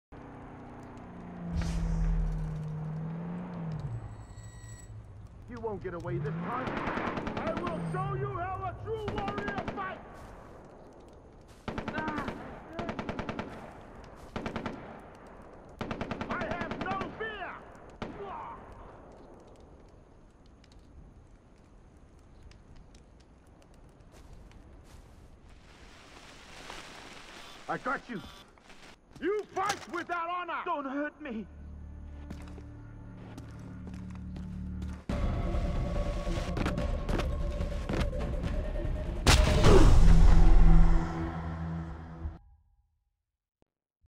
Enraged Civilians plan a massive attack on Rakyat‎ forces *CAUGHT ON FILM* [EXTREMELY GRAPHIC]
Footage sent to us autonomously, it appears the one recording did not make it, may he rest in peace.